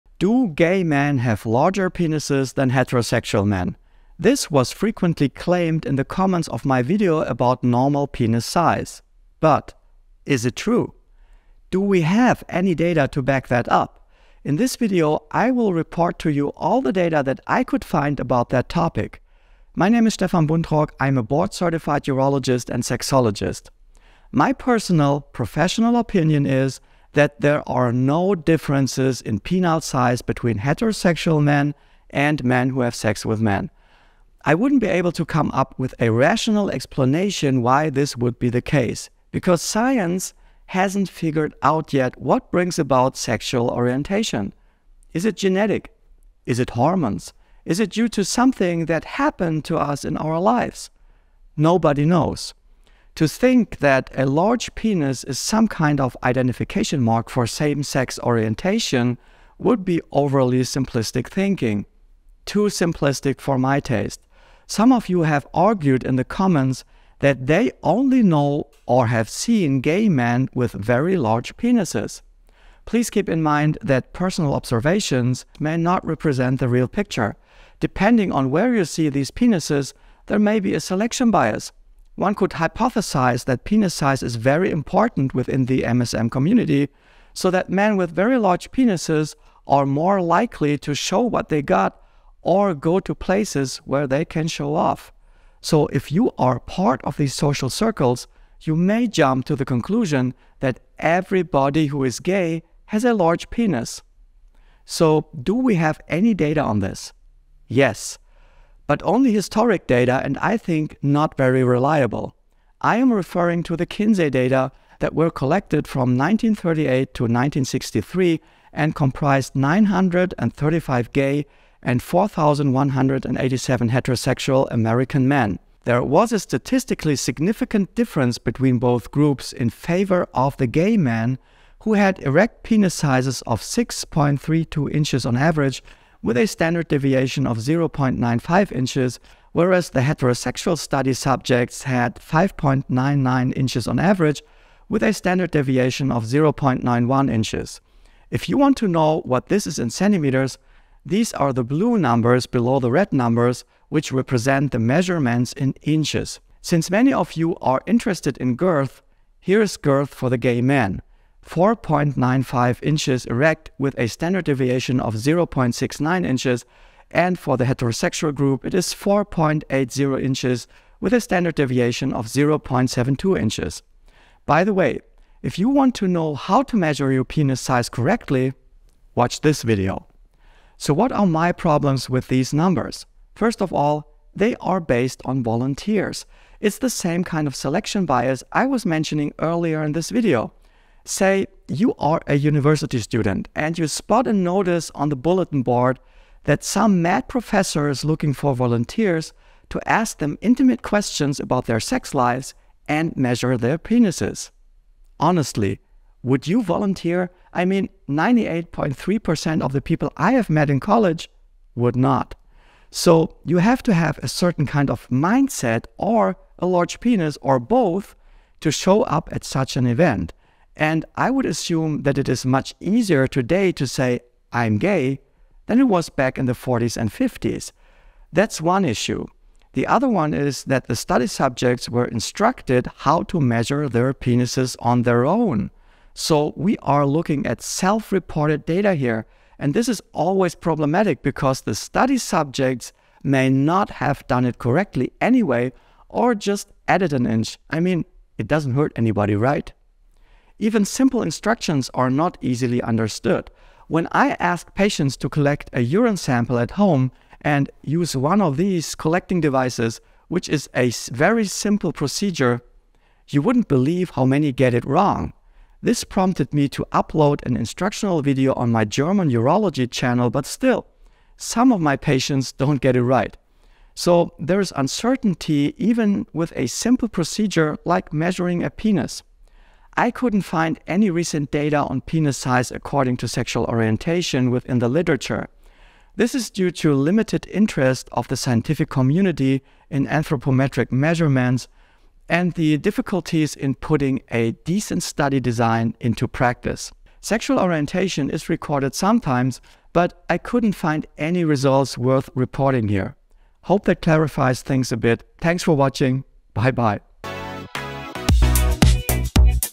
Size Wars: Gay Men vs. Heterosexual Men – Who Wins? | UroChannel
In this UroChannel video, Dr. med. Dr. phil. Stefan Buntrock, a board-certified urologist and sexologist from Göttingen, addresses the claim that gay men have larger penises than heterosexual men. Buntrock presents the data he found on this topic, stating that there are no differences in penile size between heterosexual men and men who have sex with men. He explains that the scientific community has yet to determine what causes sexual orientation, whether it is genetics, hormones, or life experiences. Buntrock dismisses the notion that penis size is an identification mark for same-sex orientation, attributing personal observations to selection bias. He discusses historic data from the Kinsey studies conducted between 1938 and 1963, which showed a statistically significant but not entirely reliable difference in erect penis sizes between gay and heterosexual men. Buntrock acknowledges the limitations of the Kinsey data, including the self-reporting nature of the measurements and the volunteer-based sample. He concludes that there is limited recent data on penis size according to sexual orientation and highlights the lack of interest and challenges in conducting such studies.